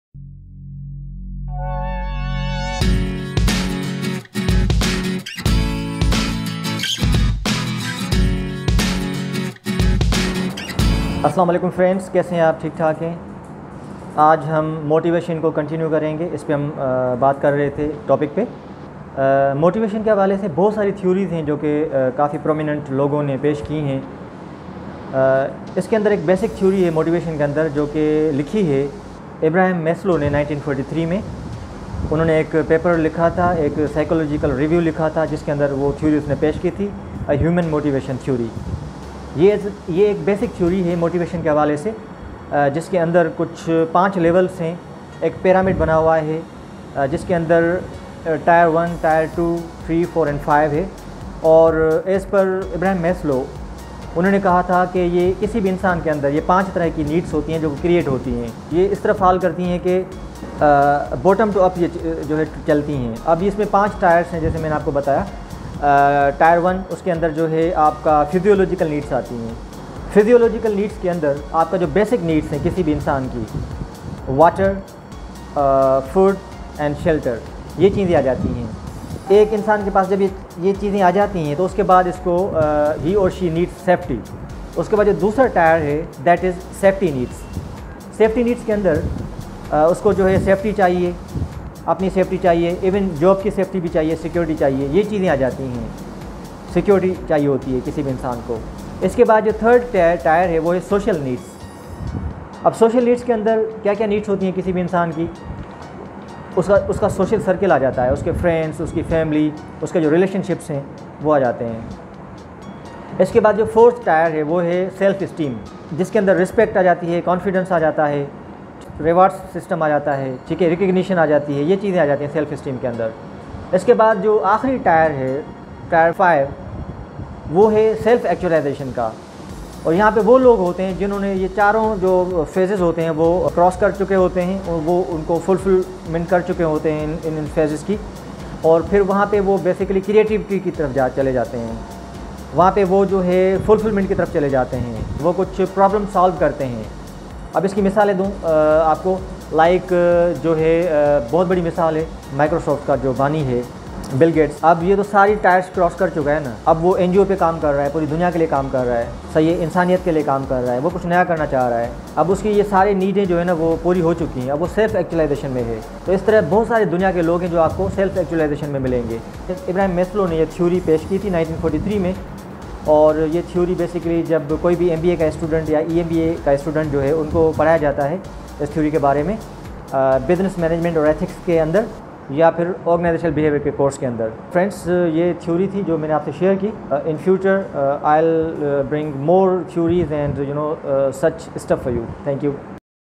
hierarchy of needs in Urdu for New marketers and MBA EMBA students
My Lecture/on Motivation Theory Maslow's hierarchy of needs in Urdu for New marketers and MBA EMBA students. Maslow's hierarchy of needs is a theory in psychology proposed by Abraham Maslow in his 1943 paper "A Theory of Human Motivation" in Psychological Review.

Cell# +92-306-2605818